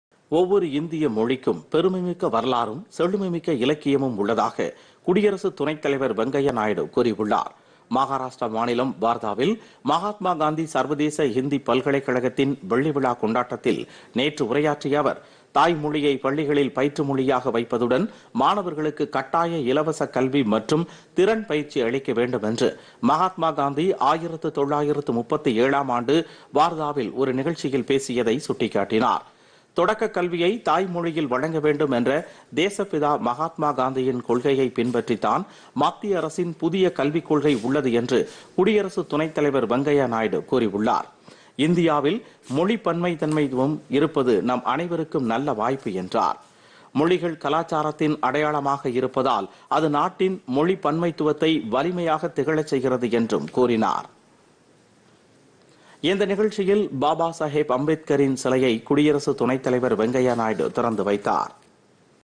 இந்திய மொழிகளுக்கு பெரு‍மைமிக்க வரலாறுPodhigaiTamilNews
ஒவ்வொரு இந்திய மொழிக்கும் பெருமைமிக்க வரலாறும், செழுமைமிக்க இலக்கியமும் உள்ளது- குடியரசு துணைத் தலைவர் வெங்கய்யா நாயுடு பெருமிதம்